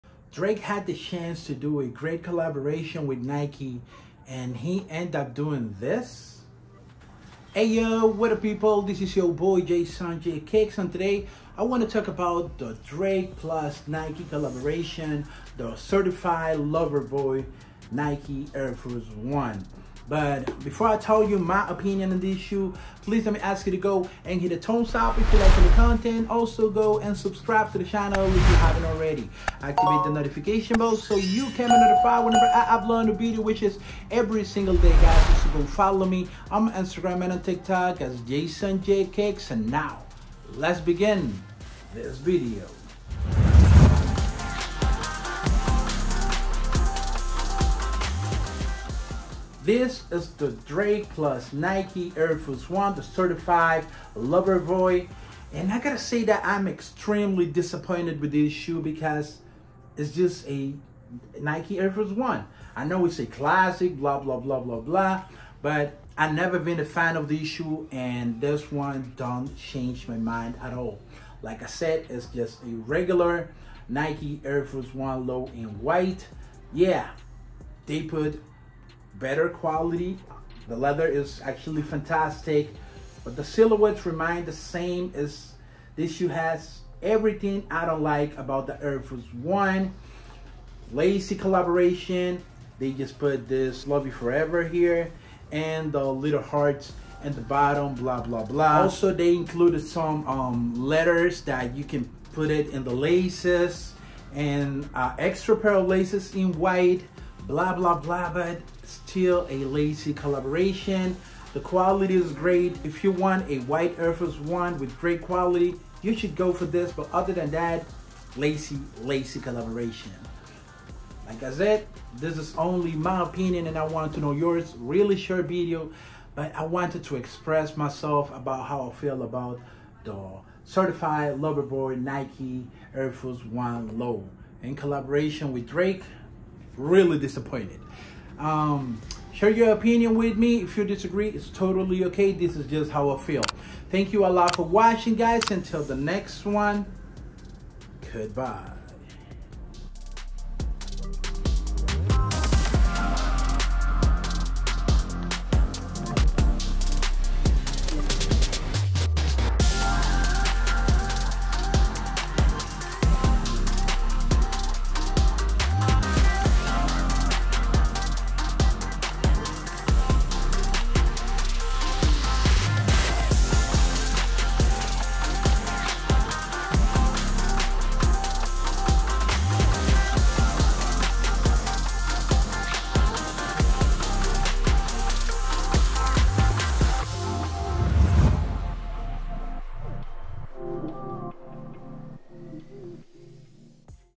Certifying Lover Boy Nike Air Force 1 Low x Drake - Review & On Feet (Worse Collab ?)
This premium leather, triple-white AF1 is a love letter to the classic model. Inspired by Drake's favorite childhood book, "Love You Forever," and the sentiment of the phrase, the intent was to enhance the quality of the shoe and add subtle details that would tell the story but wouldn't change the iconic silhouette. For the first time, the midsole is revised to read "Love You Forever" with complementary hearts that replace the stars on the outsole. Additional design elements include alphabetical beads to customize the lace dubrae, NOCTA logo in place of Nike Air on the left heel and a new perforation pattern on the toe box.